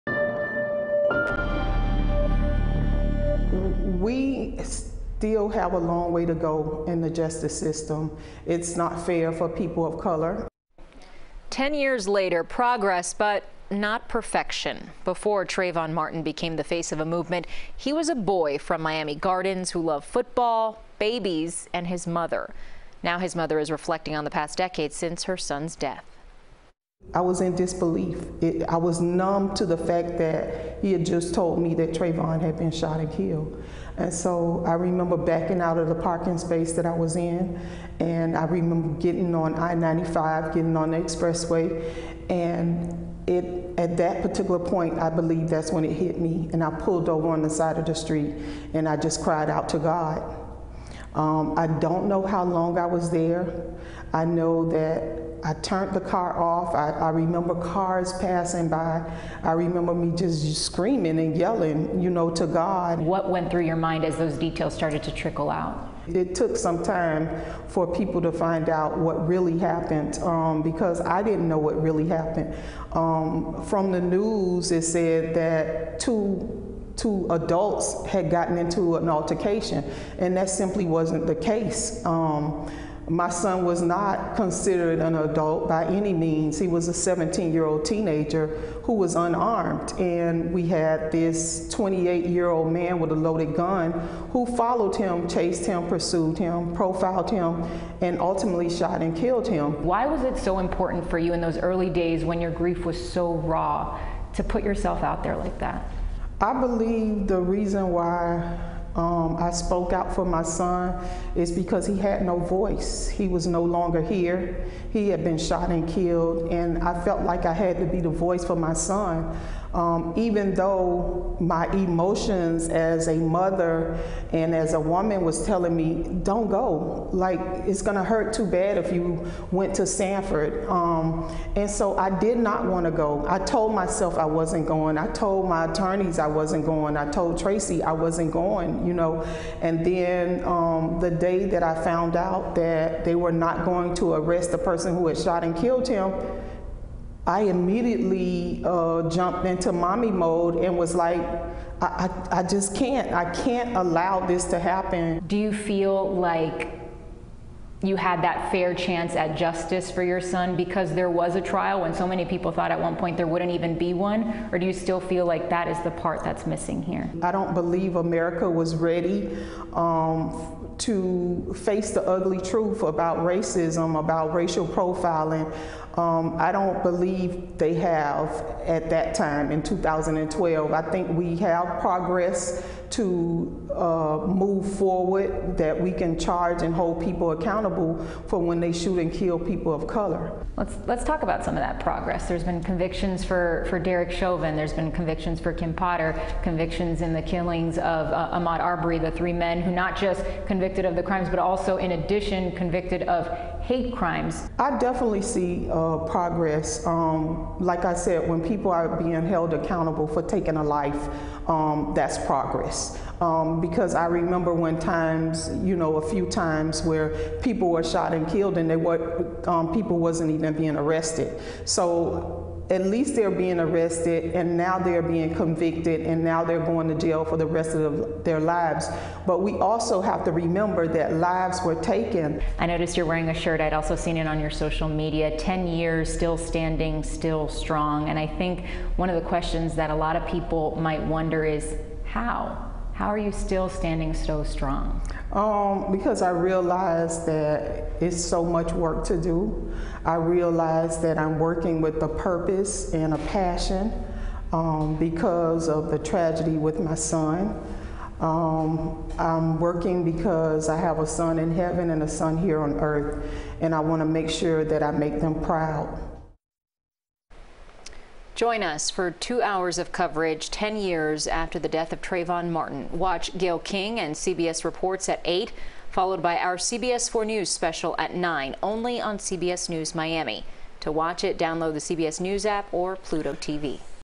Trayvon Martin 10 Years Later: Sybrina Fulton Reflects On Son's Life & Legacy
CBS4's Lauren Pastrana spoke one-on-one with Fulton.